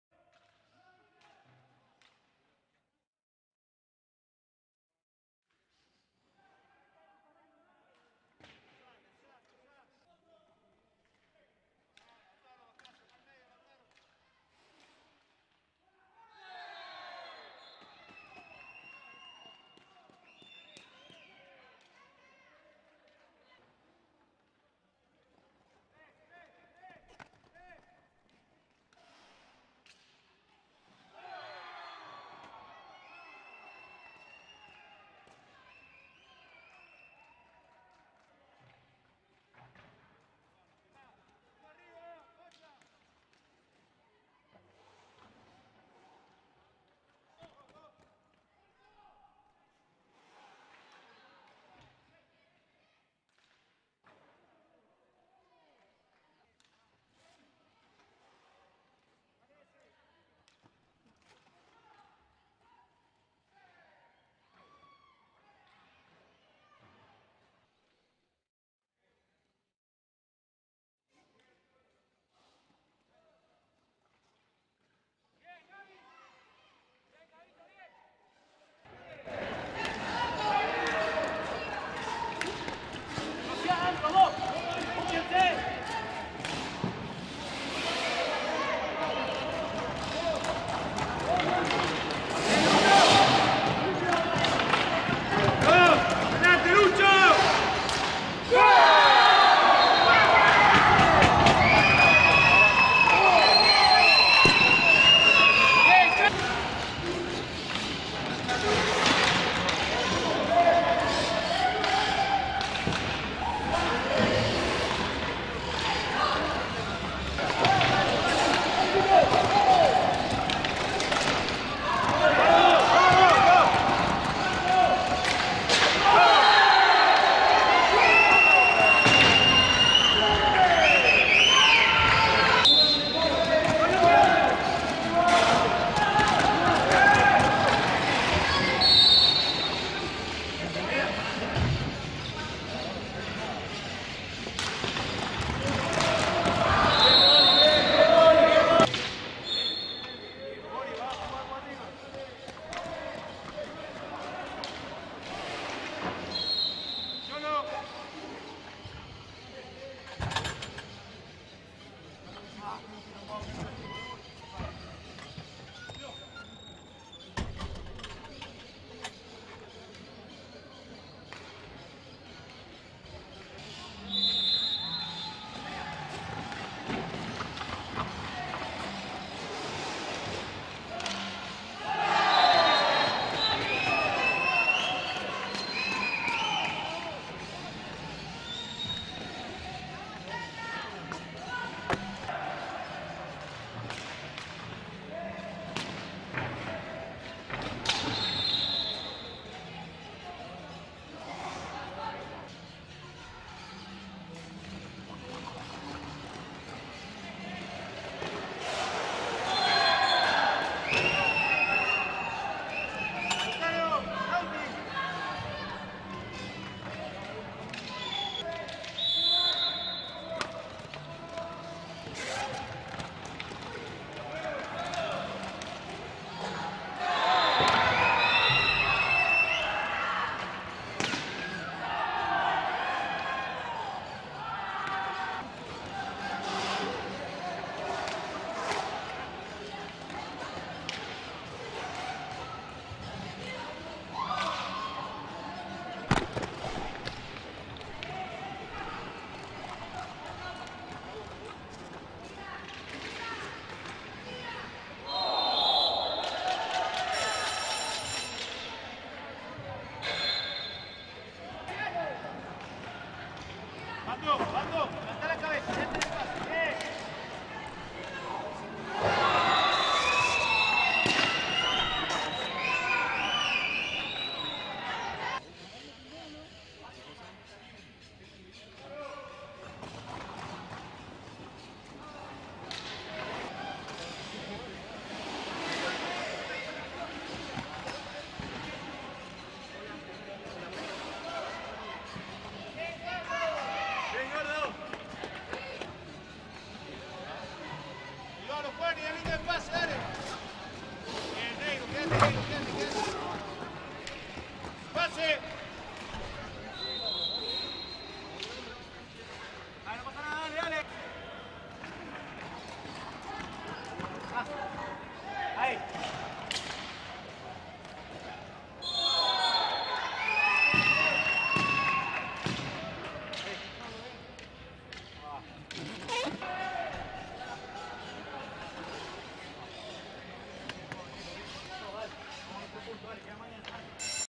RESUMEN DEL GEBA 2-9 SAN MARTIN - CAMPEONATO ARGENTINO SENIOR 2021 - FECHA 2
Resumen del partido n°9 del campeonato argentino senior 2021 disputado en Mendoza, Murialdo. Resumen del encuentro donde los leones ganaron por una amplia diferencia a Geba de Bs. As. Ambos equipos conforman el grupo del local Murialdo y del sanjuanino Bancaria.
Le queríamos agradecer a 2M AL MUNDO por brindarnos las transmisiones de todos los partidos del campeonato argentino senior 2021.
Foto: Hockey Apasionado